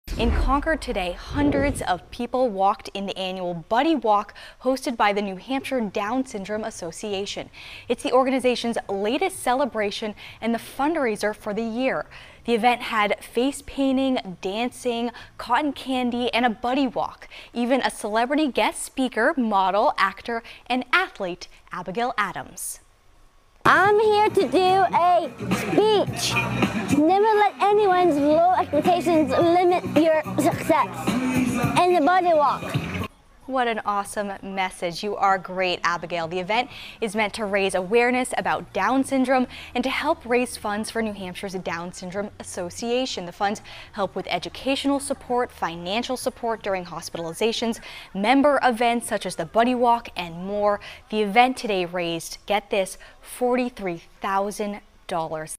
New Hampshire Down Syndrome Association hosts annual Buddy Walk
The NHDSA's Buddy Walk is one of the organizations latest celebrations and fundraisers for the year. Abigale Adams was a celebrity guest.